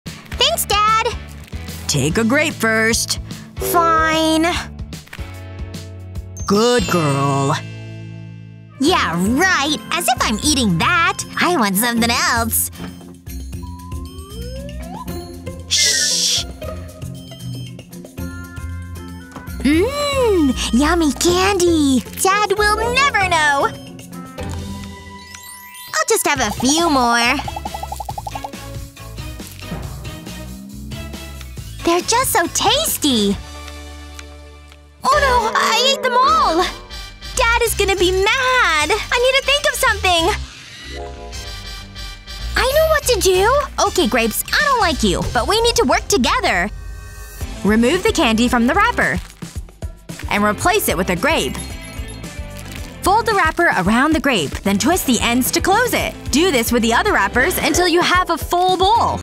AWESOME HACKS FOR CHOCOLATE LOVERS || Funny Pranks For Parents by 123 GO! Kevin
When your stomach starts to rumble, then you need some clever food hacks!  ⭐️
What was your favourite?